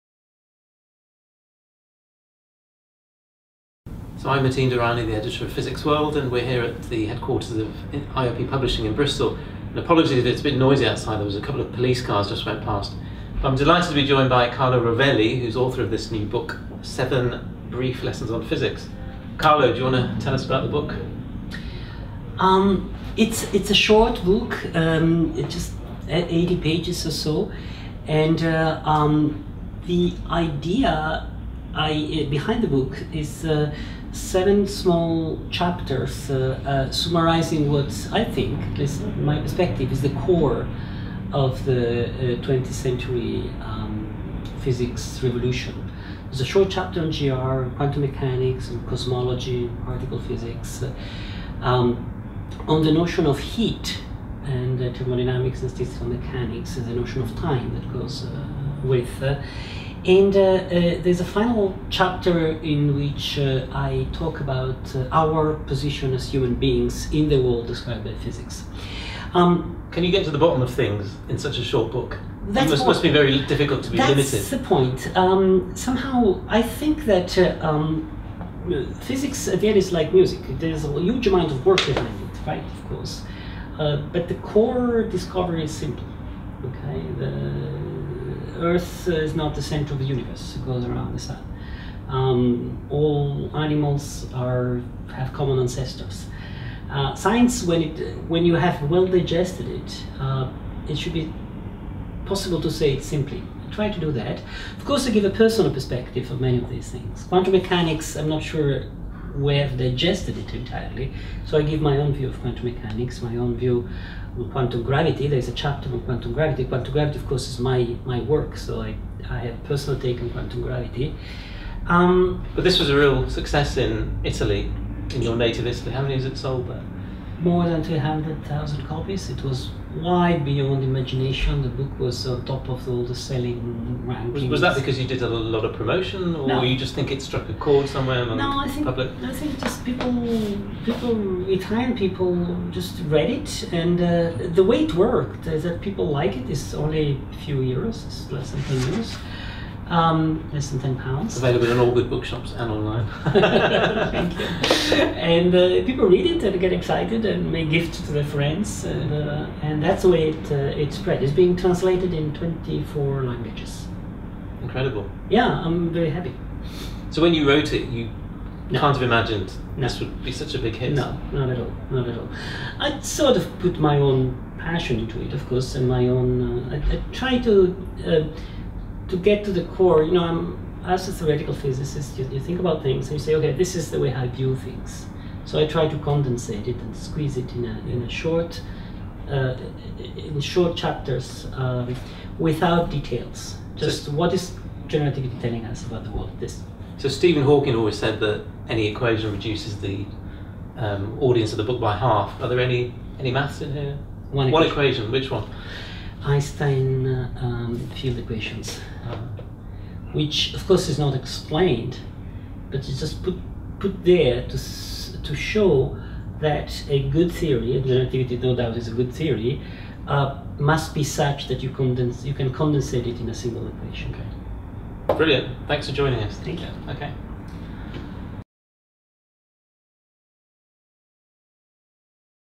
Carlo Rovelli discusses his new book 'Seven Brief Lessons on Physics'
Theoretical physicist and popular-science writer Carlo Rovelli discusses his new book 'Seven Brief Lessons on Physics' with the editor of Physics World Matin Durrani.